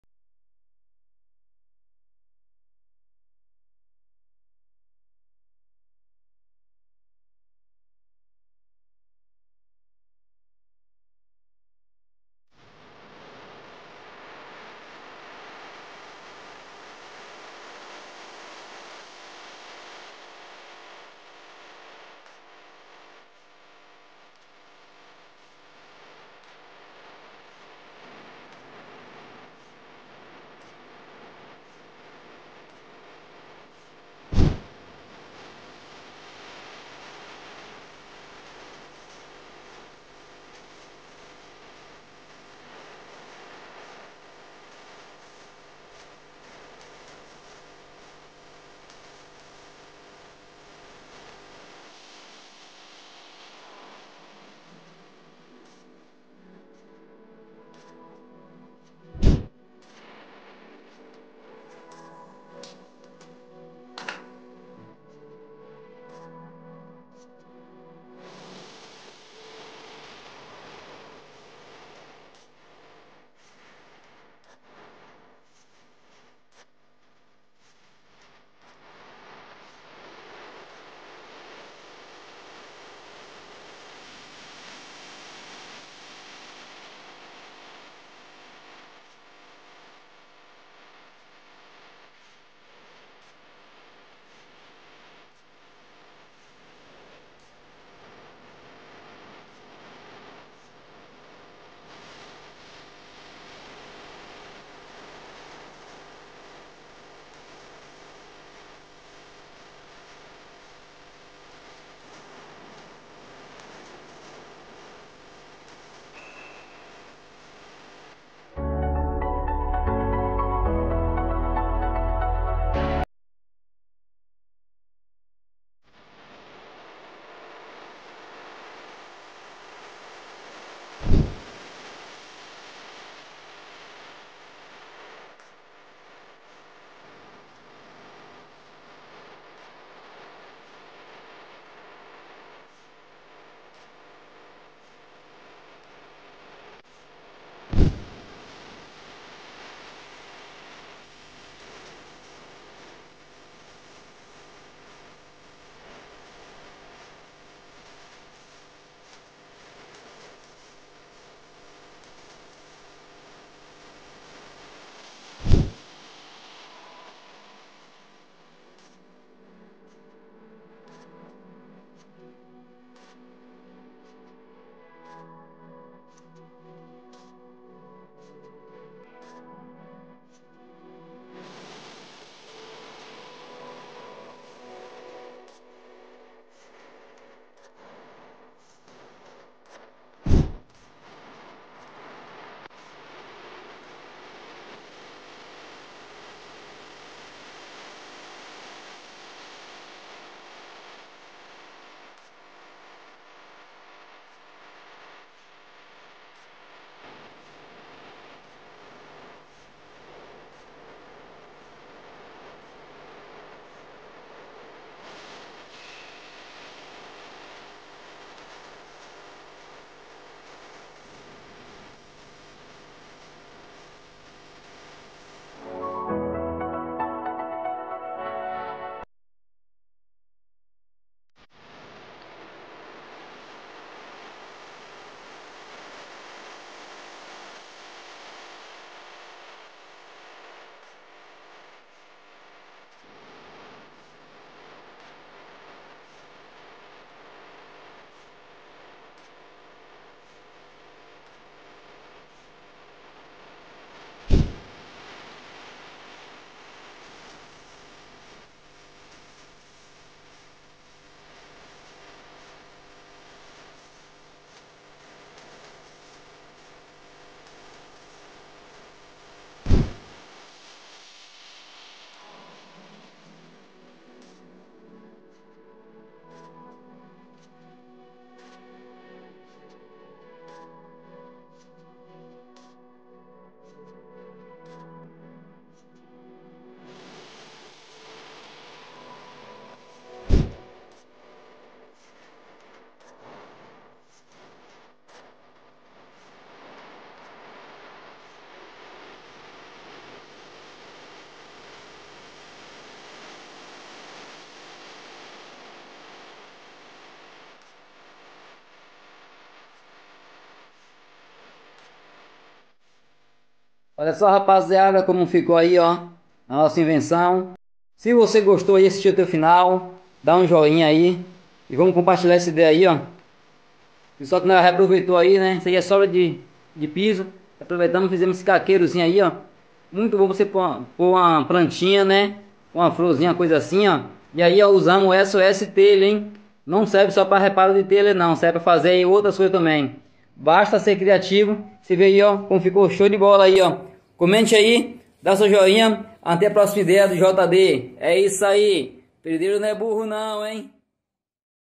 PEDREIRO NÃO PODE SER ESTUDANDO TEM SER ANALISANDO
esse pedreiro fez isso usando apenas restos de piso que tinha em casa! e ficou lindo demais faça você também